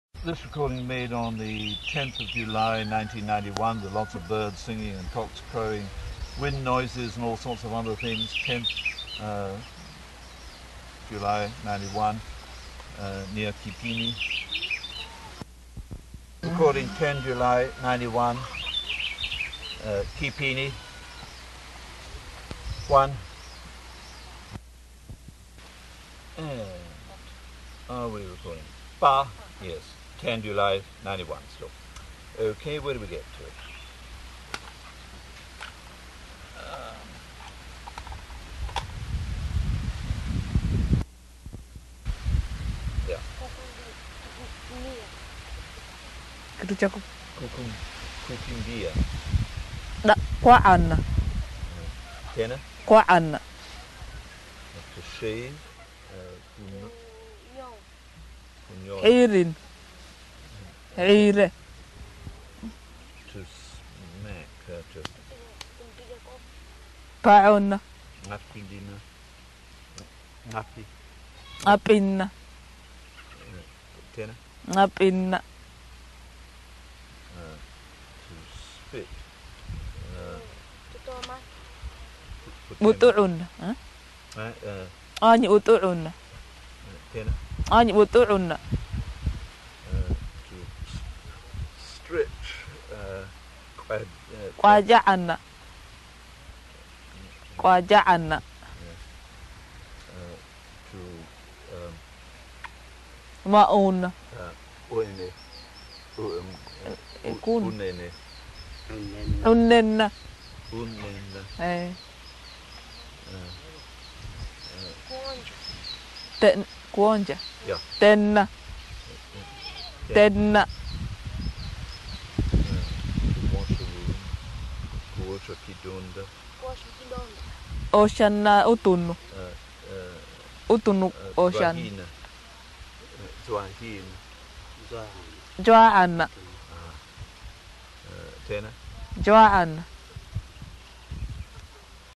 Dahalo audio: dal_word-list_1991_47
Dahalo (dal) audio: dal_word-list_1991_47
Language family: Afroasiatic; Erythraean; Cushitic; South Cushitic;
Kipini, Kenya
Date: 7/10/1991

Description: Language Dahalo.  dal_word-list_1991_04.html, enties: 136- 143

Contributor: Dibili
UCLA Phonetics Archive